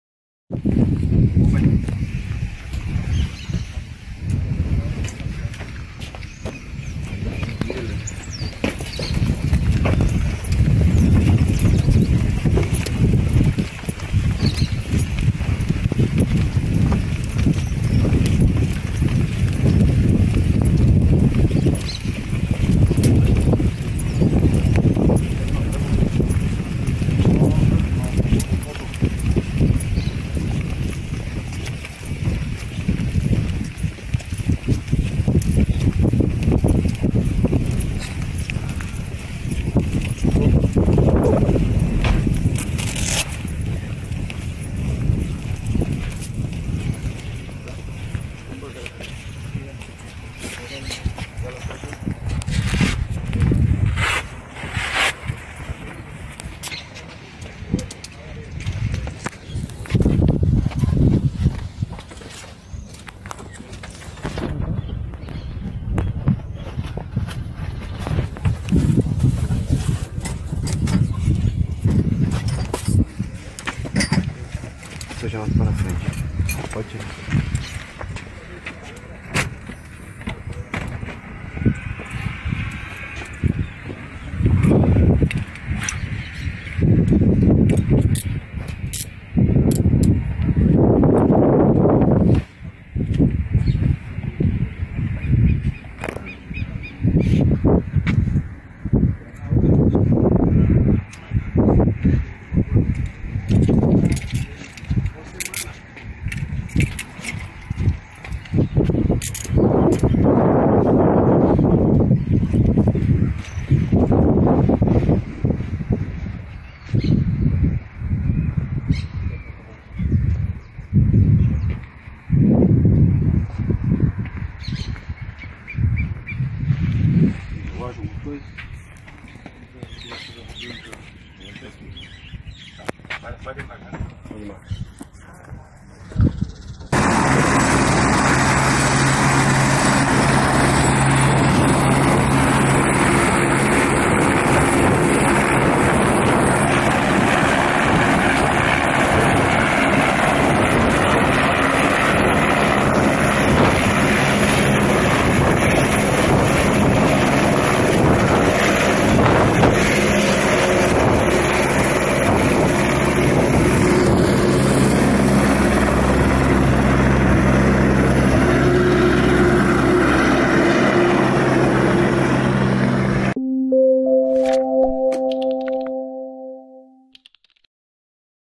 Narcotraficante Marcelo Piloto é extraditado do Paraguai para o Brasil
Narcotraficante Marcelo Piloto é entregue às autoridades brasileiras após ser expulso do Paraguai. o avião com o traficante partiu do grupo tático aéreo da Força Aérea Paraguaia, em Luque, cidade vizinha a Assunção, às 5h05 desta segunda-feira.